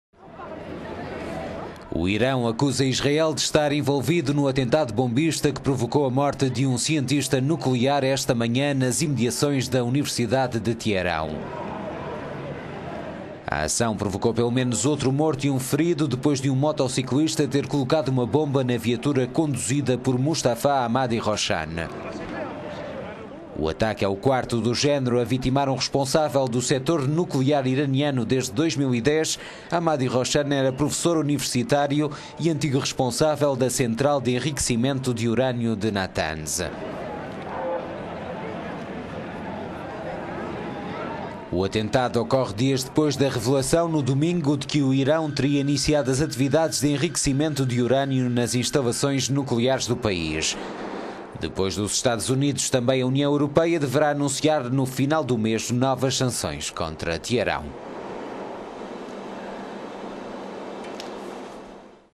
Irão acusa Israel de autoria do atentado contra cientista nuclear
O Irão acusa Israel de estar envolvido no atentado bombista que provocou a morte de um cientista nuclear, esta manhã, nas imediações da Universidade de Teerão.

A ação provocou pelo menos outro morto e um ferido depois de um motociclista ter colocado uma bomba na viatura conduzida por Mostafa Ahmadi Roshan.

O ataque é o quarto do género a vitimar um responsável do setor nuclear iraniano desde 2010.

Ahmadi Roshan era professor universitário e antigo responsável da central de enriquecimento de urânio de Natanz.